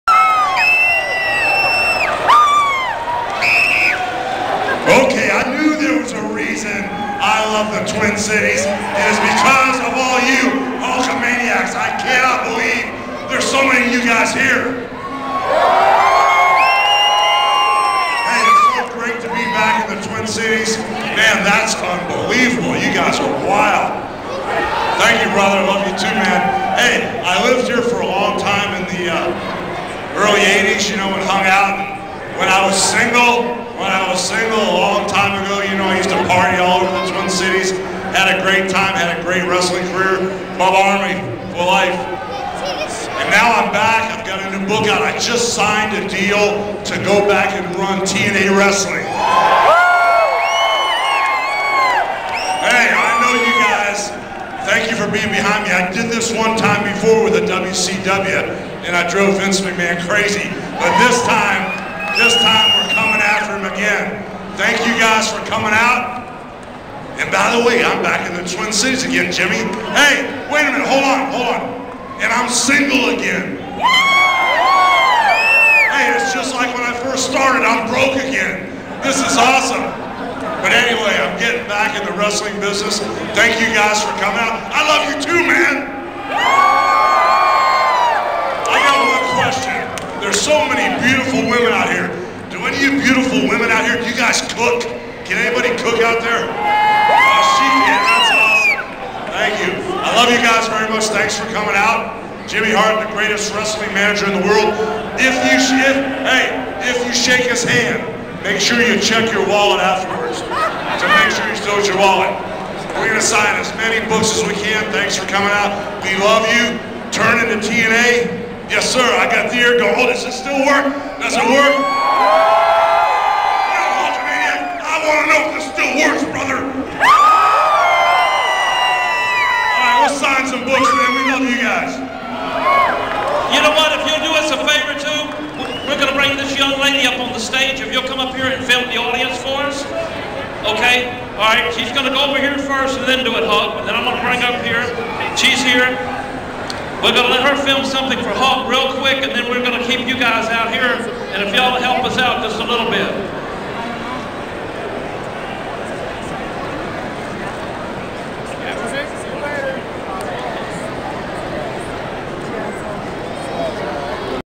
Meeting the Hulkster @ MOA in Minnesota
got in line to get him to sign his book - My Life Outside The Ring